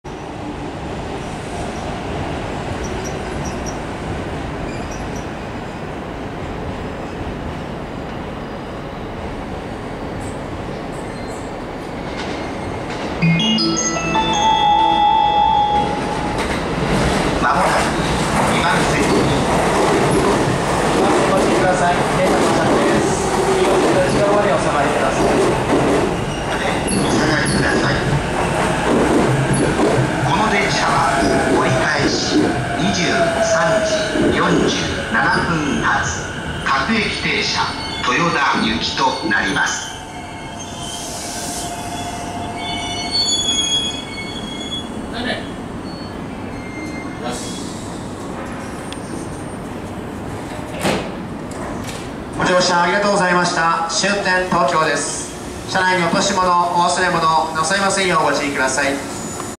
JR East E233-0 Series arriving Tokyo
[Local: Tokyo » Toyoda]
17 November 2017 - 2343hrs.

JR東日本E233系0番台東京到着
[各駅停車：東京 » 豊田]
2017年11月17日　23時43分